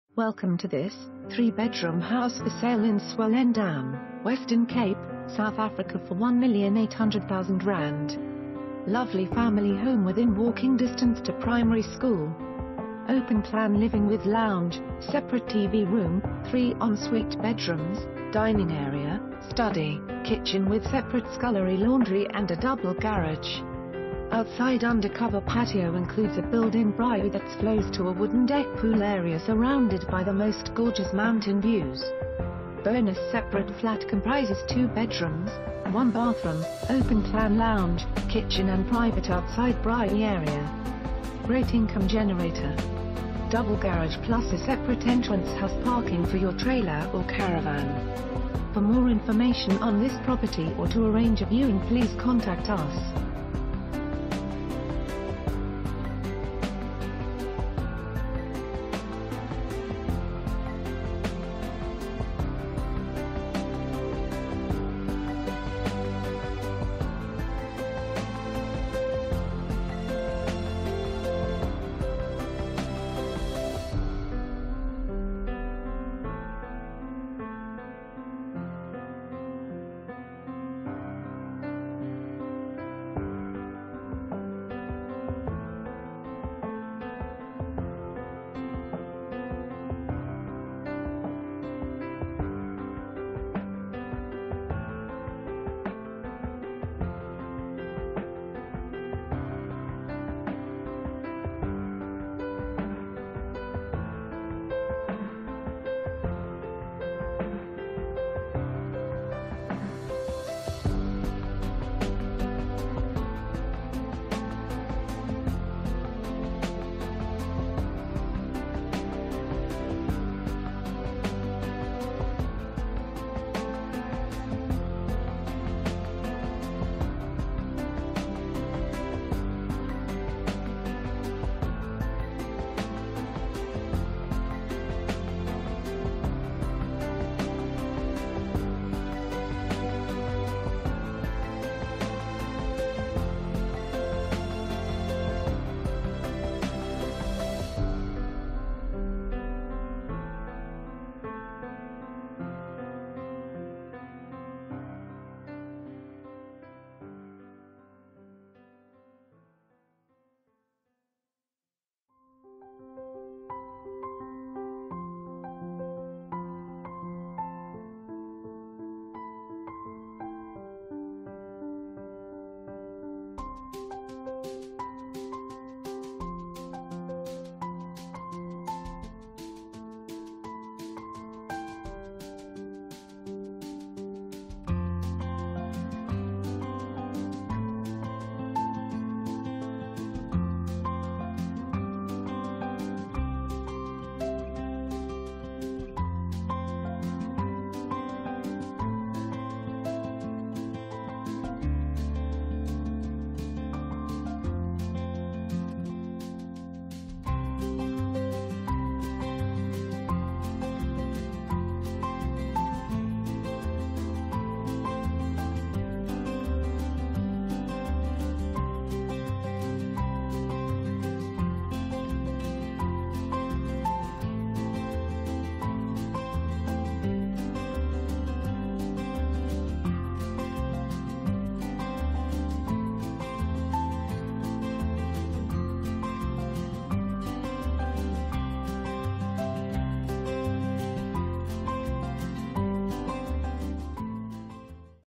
3 Bedroom House For Sale in Swellendam, Western Cape, South Africa for ZAR 1,800,000
Lovely family home within walking distance to primary school.  Open plan living with lounge, separate tv room, 3 en-suite bedrooms, dining area, study, kitchen with separate scullery/ laundry and a double garage. Outside under-cover patio includes a build-in braai thats flows to a wooden deck pool area surrounded by the most ...